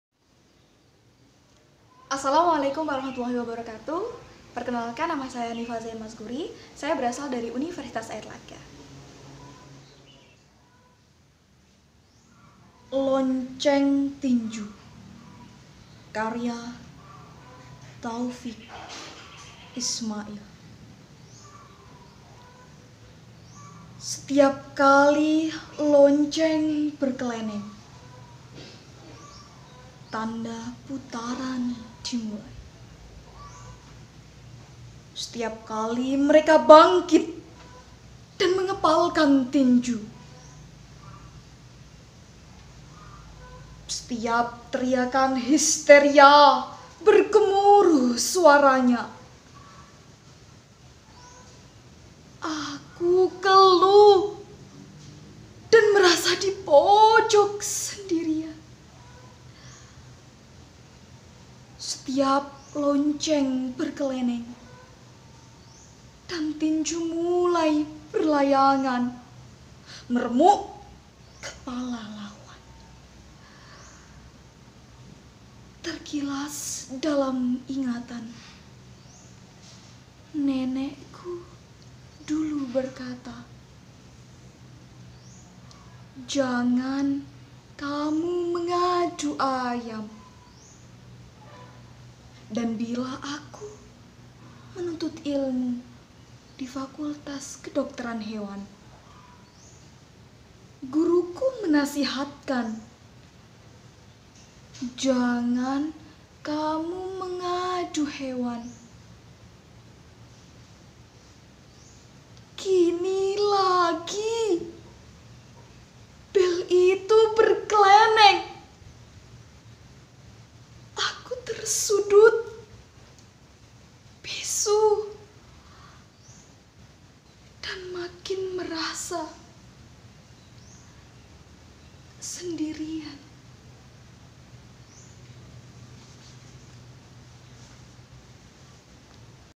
Lonceng Tinju Taufiq Ismail || POSESIF2020_BACAPUISI Hanifah Zayyan Maskuri
Assalamuallaikum Shobat

Lomba baca puisi POSESIF pekan olahraga seni BEM FMIPA UM 2020.